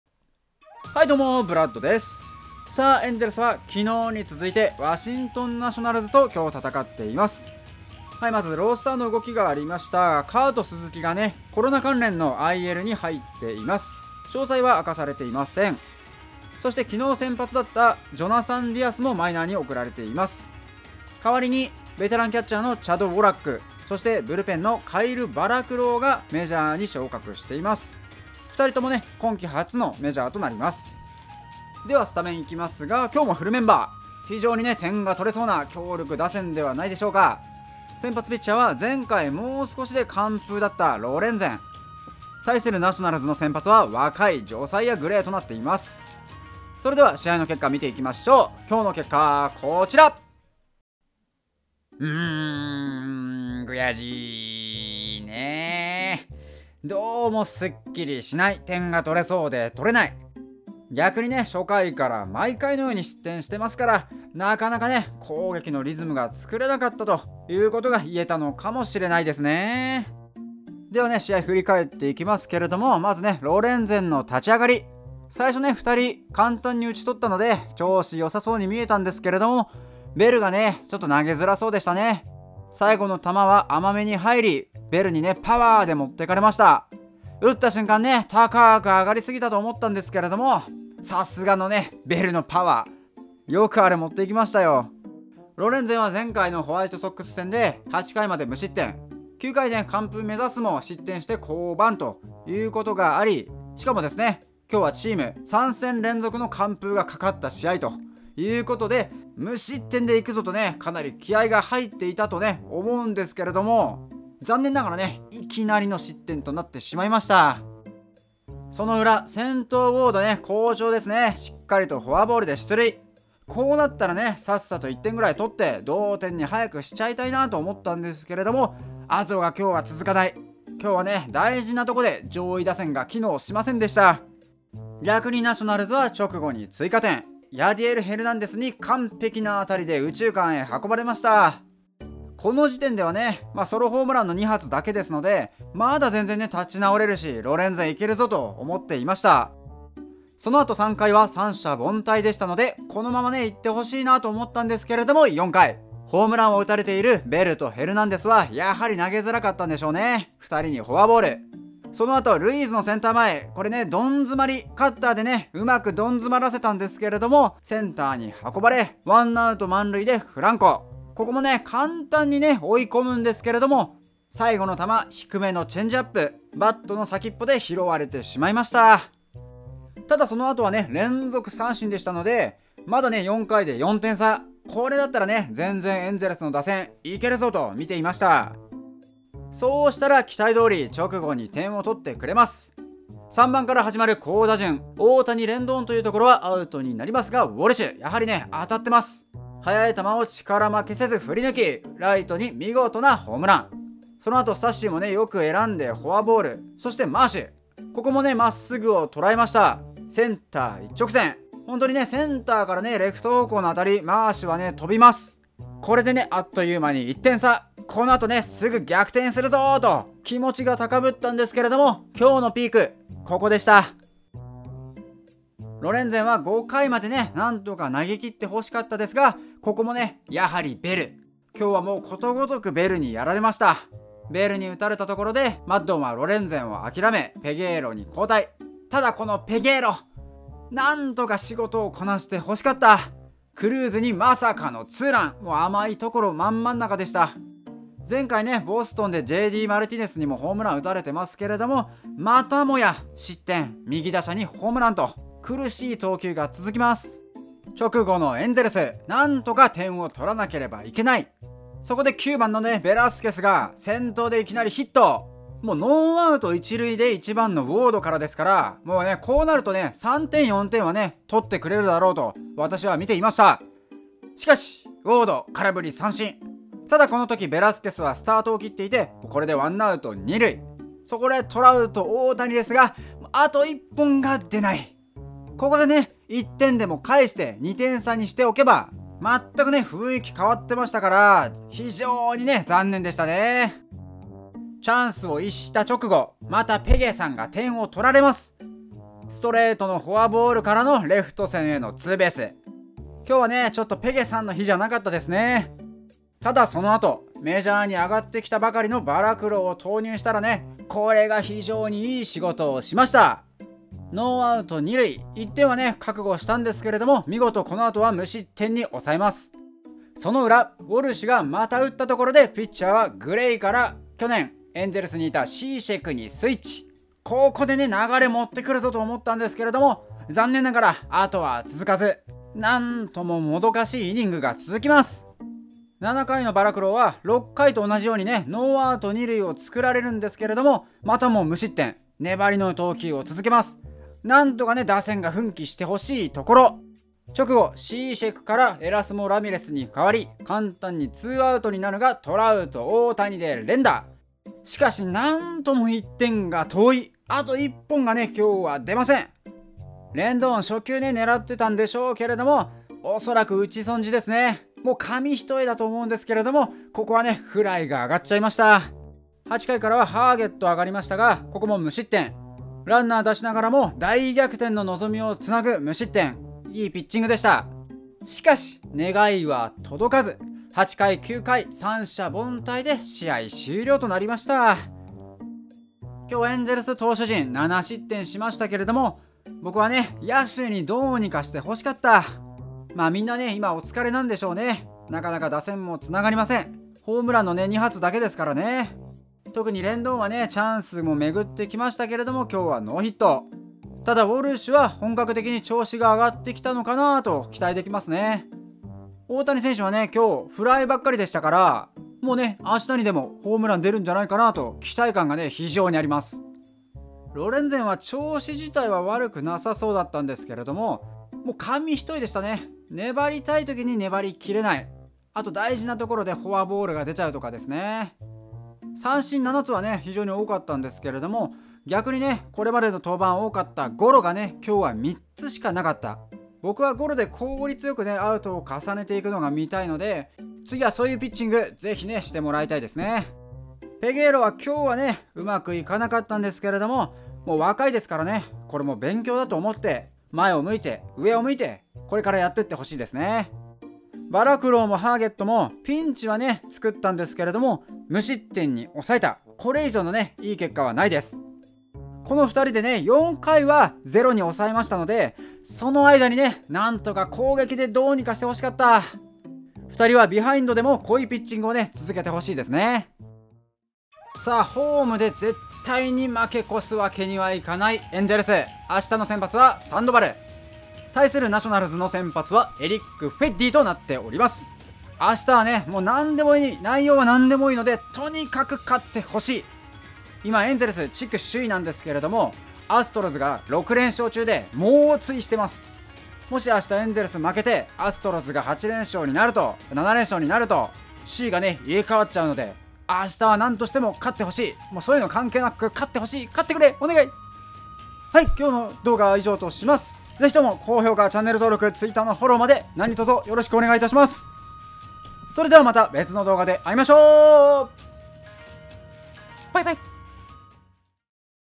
【ペゲー‼#28】ロレンゼン不調！打線お疲れの模様！エンジェルス3連続完封ならず！バラクロー・ハーゲットよく投げた！ トラウト エンゼルス ナショナルズ ソト 大谷翔平 メジャーリーグ MLB
疲れたときには勝利が一番‼

20連戦が終わるまであと5戦。

明日気持ちよく勝って、強豪レイズに挑みたい👊👊👊

エンゼルスのことを中心にメジャーリーグのことを発信していきます。